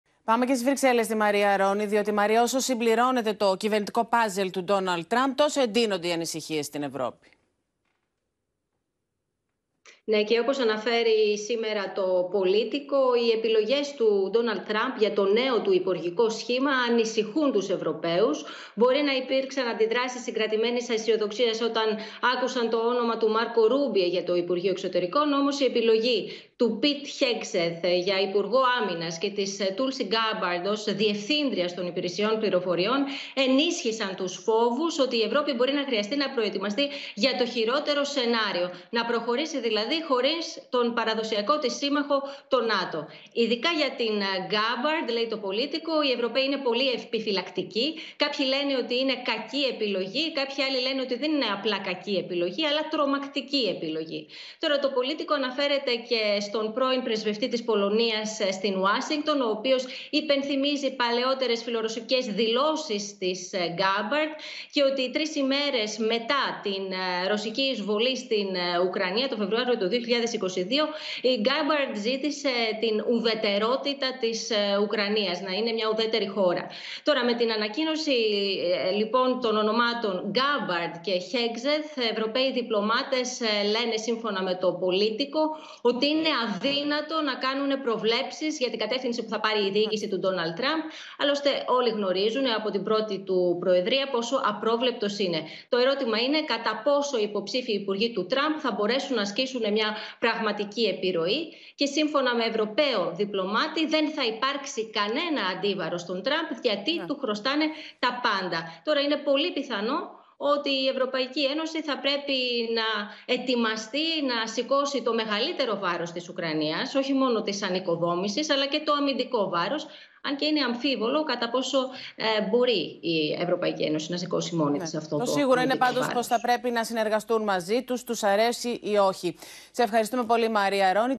Politico: Σοκ στην Ευρώπη για τους νέους επικεφαλής στο υπ. Άμυνας και της CIA | OPEN TV
Σύμφωνα με ευρωπαίο διπλωμάτη που μίλησε στο Politico, τόνισε πως «τώρα η Ευρώπη θα αναλάβει μεγαλύτερο βάρος για την Ουκρανία».